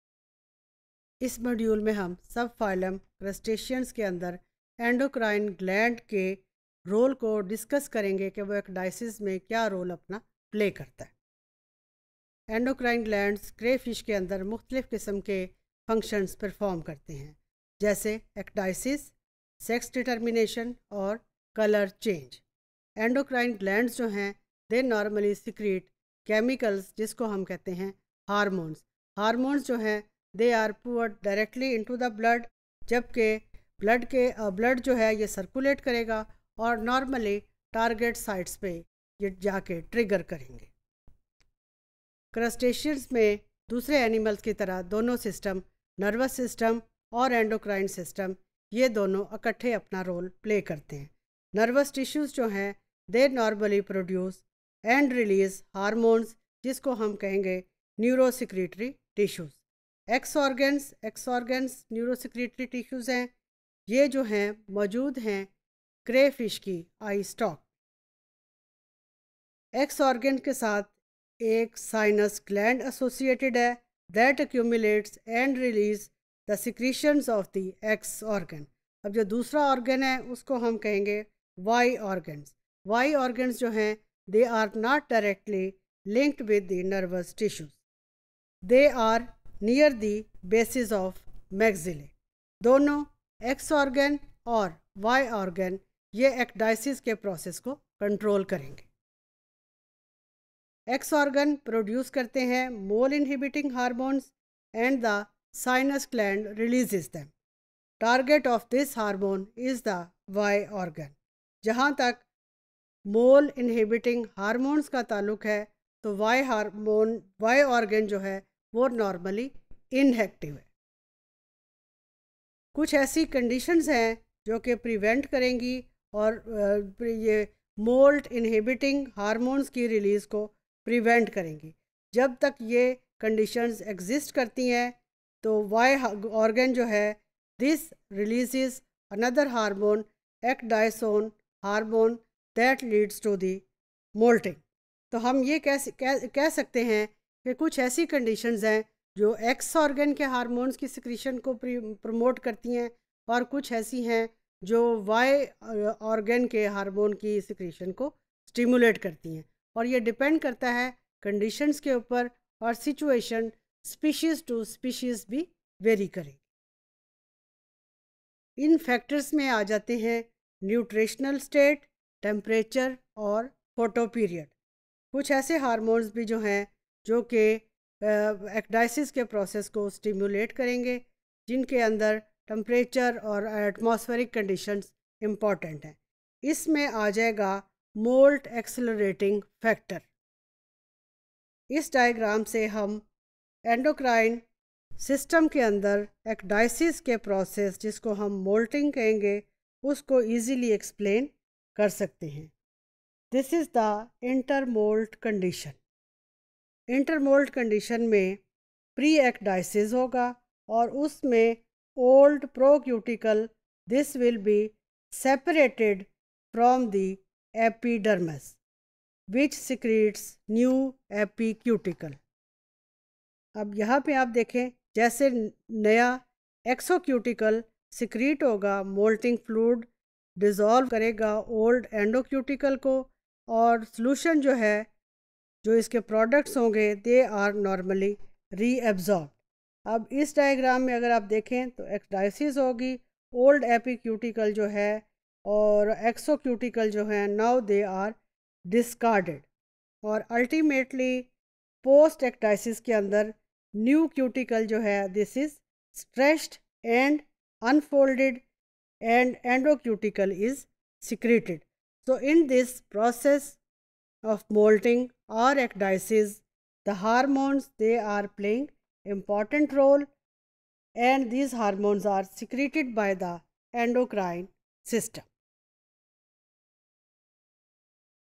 Role of Endocrine System in Ecdysis of Crustacea | Animal Diversity Invertebrates | ZOO512T_Topic153
Topic153: Role of Endocrine System in Ecdysis   of Crustacea